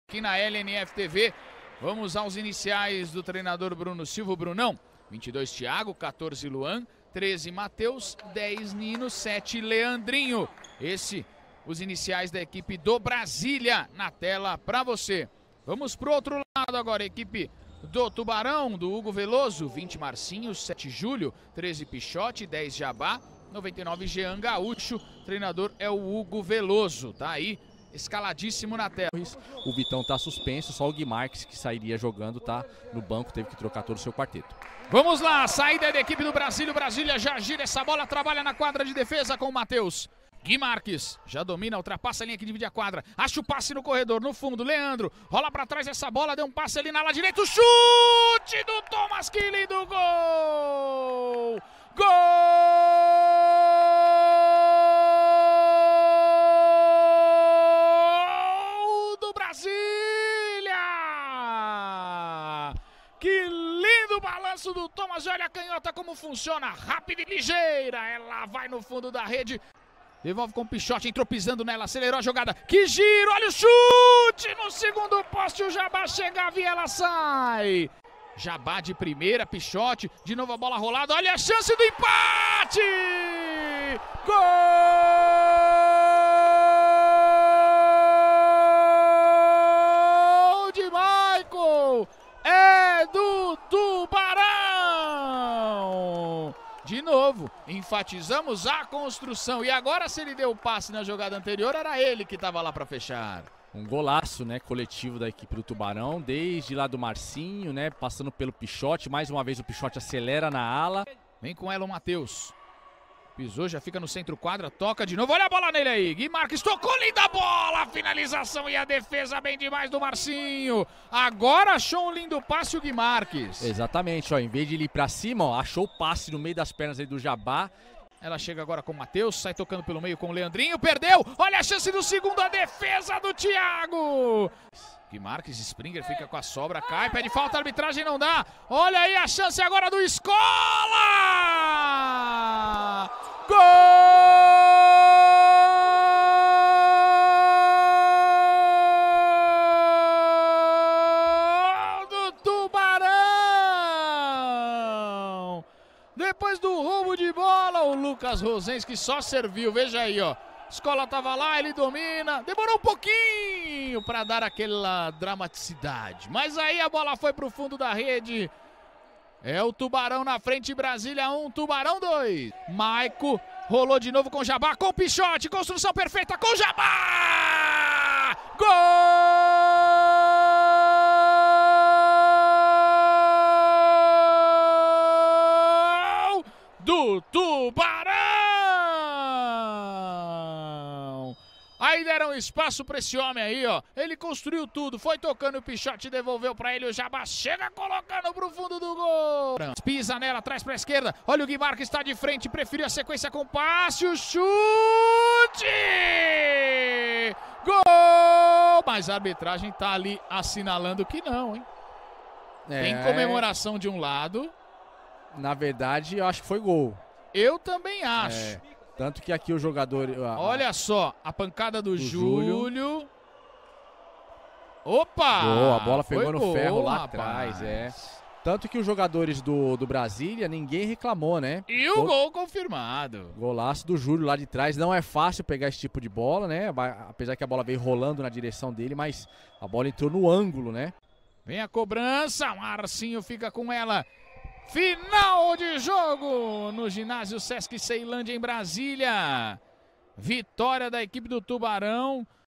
LNF2021 - Melhores Momentos - Brasília 1 x 4 Tubarão
Gols:
Brasília - Thomas
Tubarão - Maico, Vini Scola, Eduardo Jabá e Júlio
Transmissão LNFTV